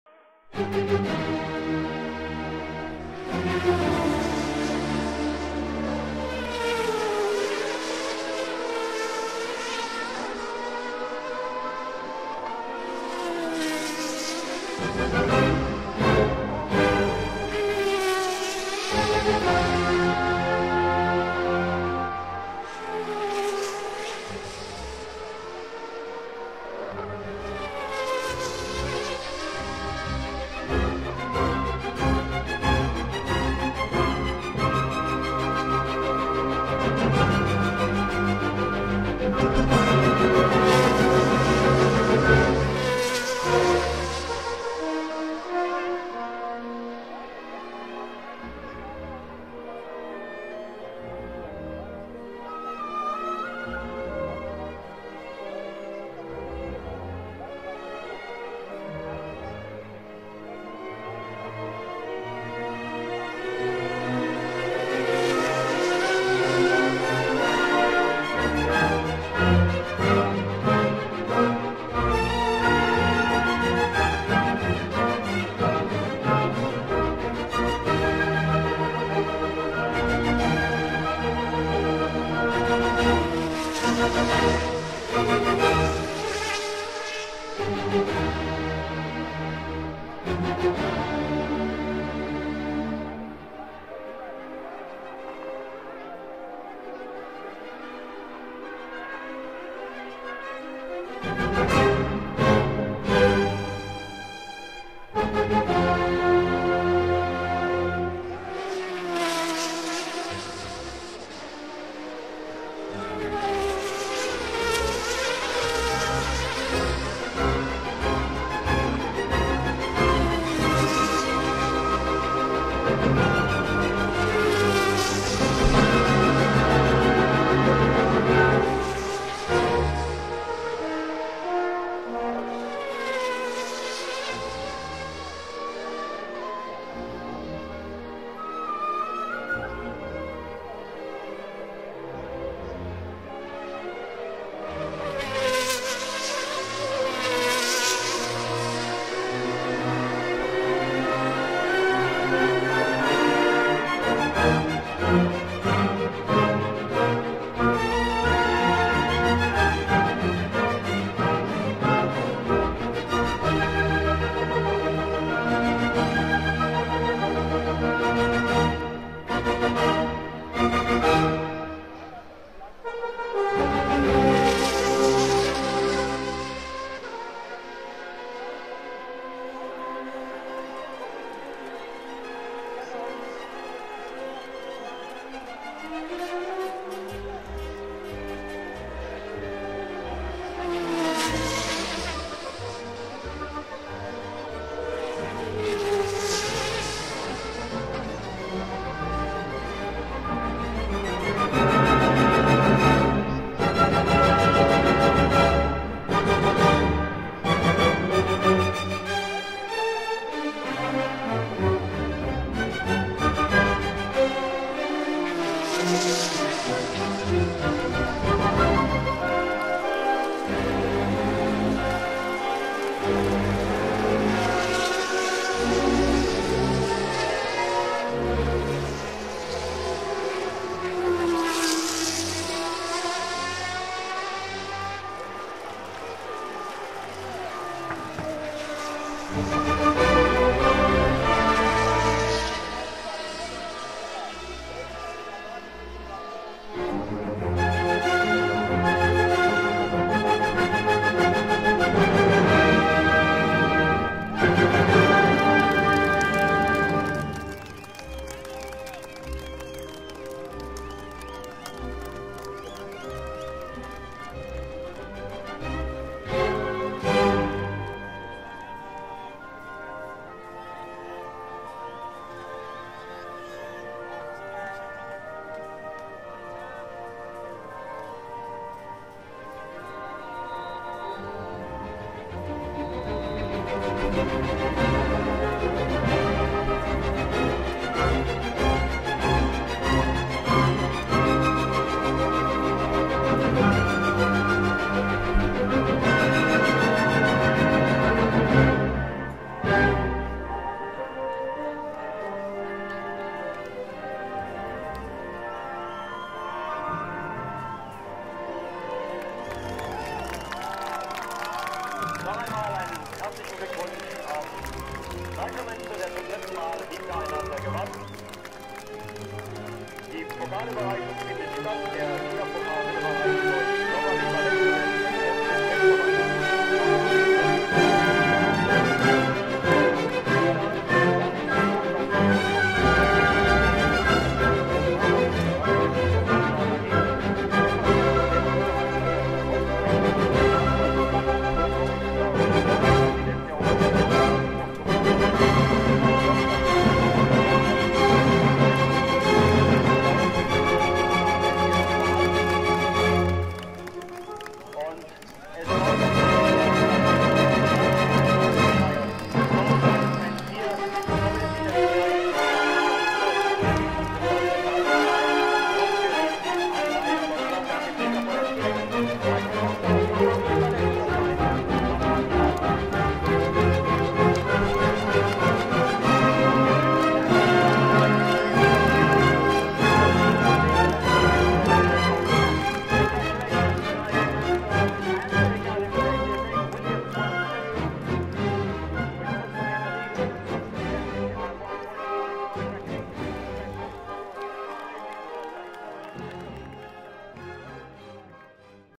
A.C.C.U. en la Cena 66º Aniversario Asociacion Argentina de Volantes
Viaje de integrantes del Auto Club Concepción del Uruguay, a la Cena del 66º de la Asociación Argentina de Volantes el 12 de Diciembre de 1997.